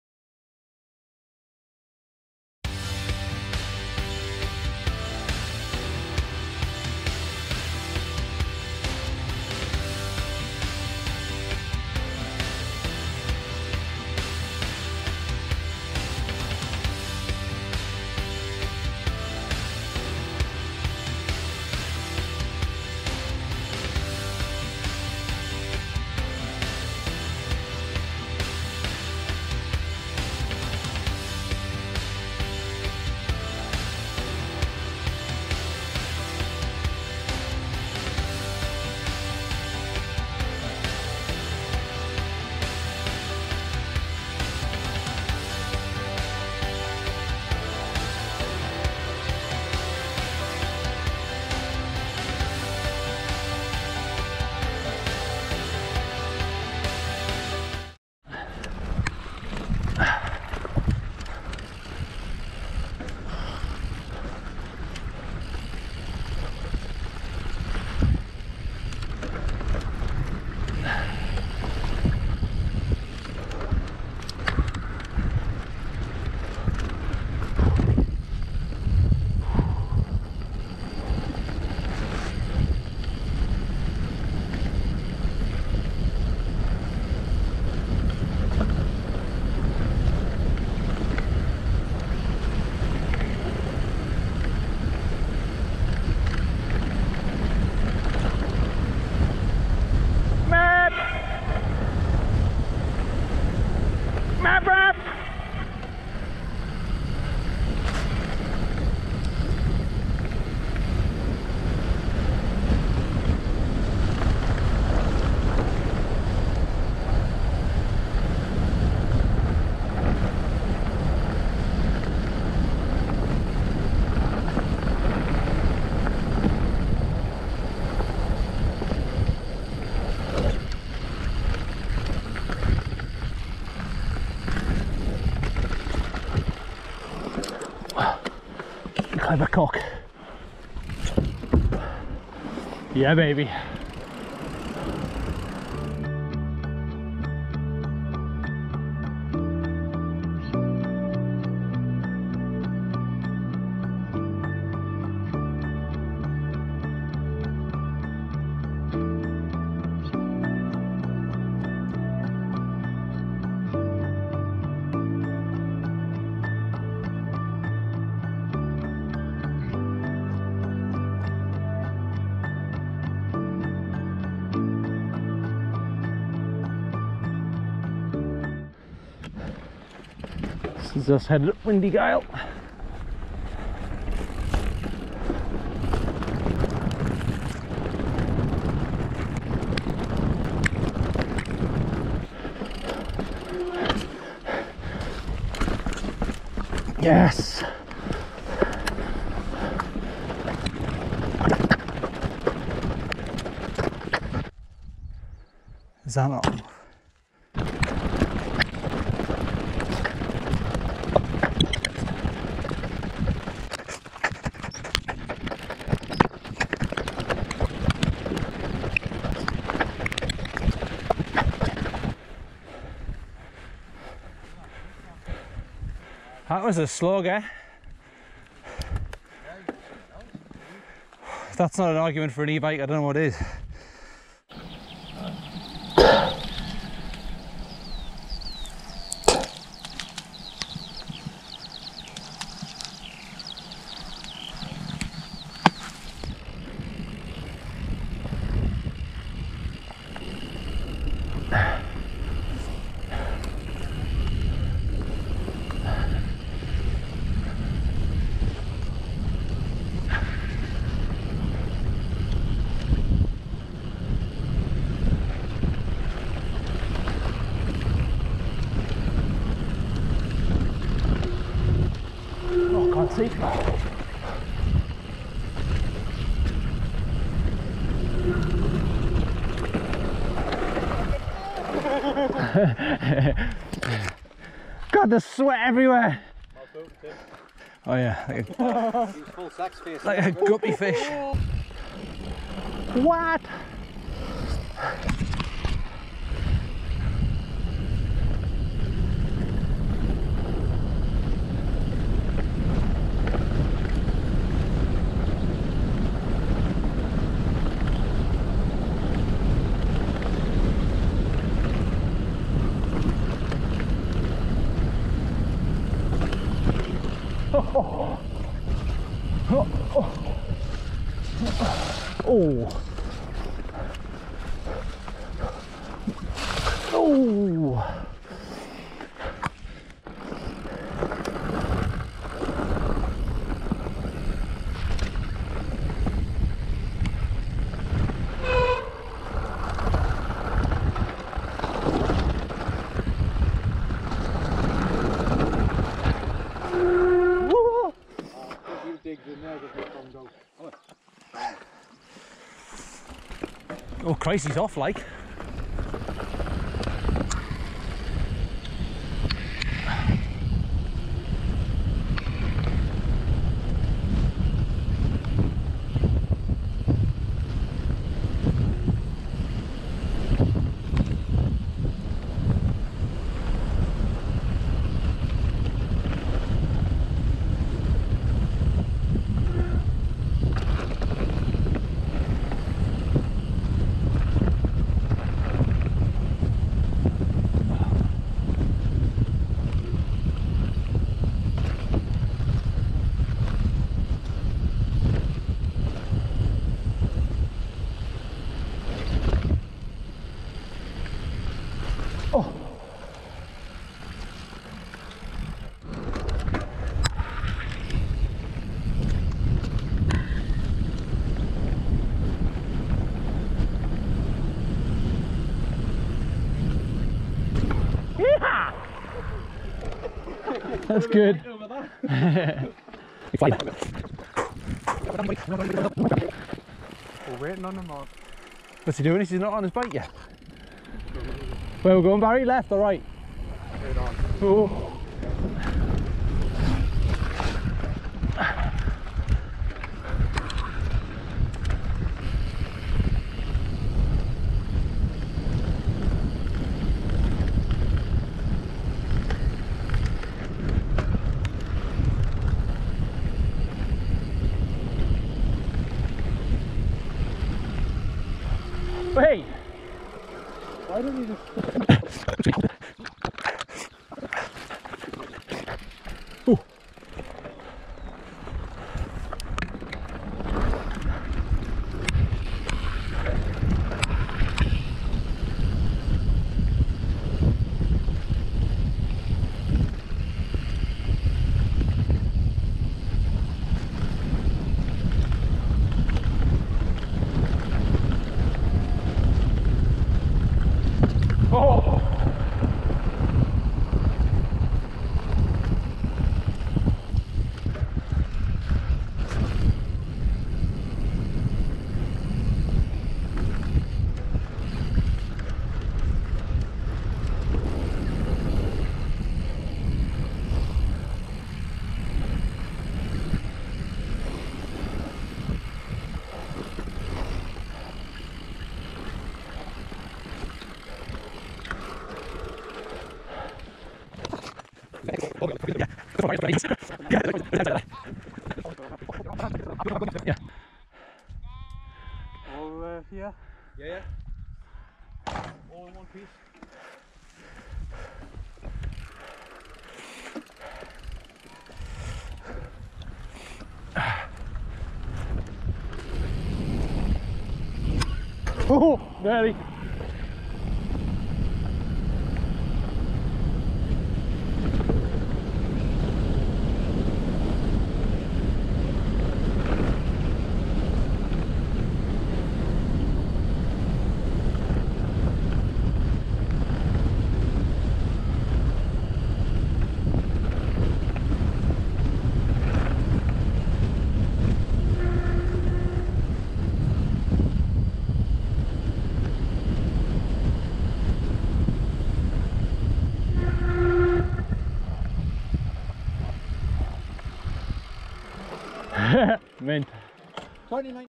A little loop - Windy Gyle
A loop round Windy Gyle chasing "the goat" and the e-bike w r. What a slog but an incredible night at the top! Orbea Rallon did well for an Enduro bike. Forgot second drone battery and editing is poor at best, will get better.